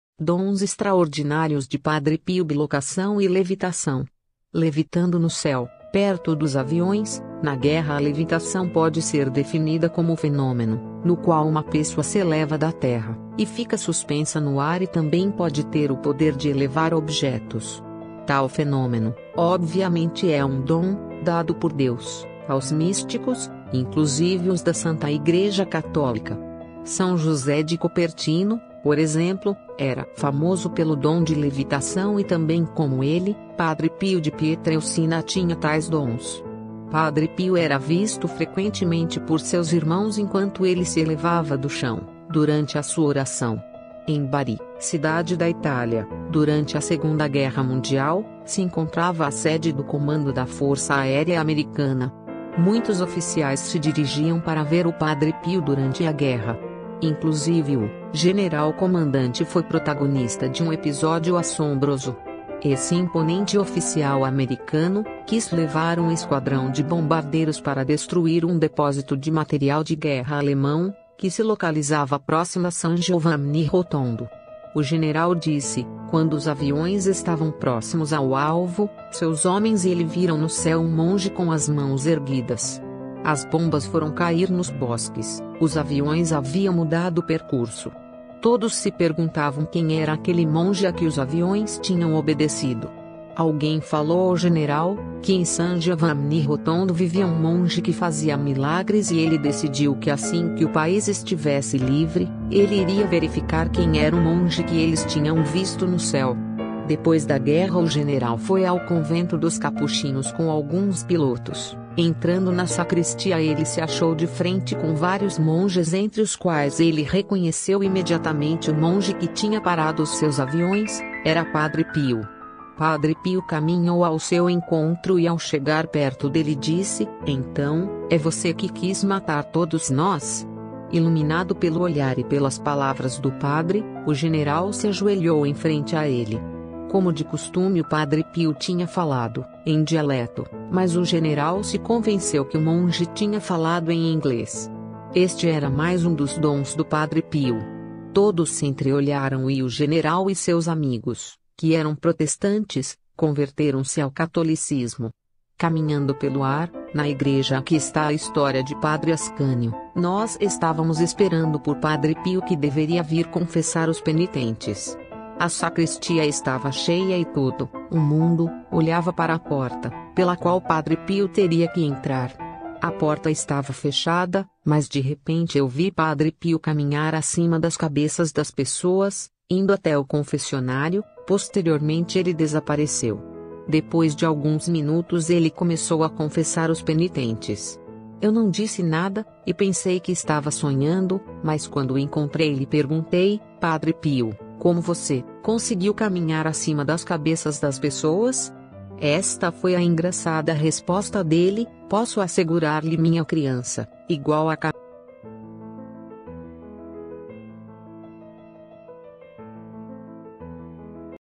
Dons extraordinários Padre Pio que levaram protestantes à converterem-se ao Catolicismo.
Dois dos maravilhosos dons concedidos por Deus a Padre Pio , um deles , a BILOCAÇÃO, levou aviadores protestantes à converterem-se ao Catolicismo.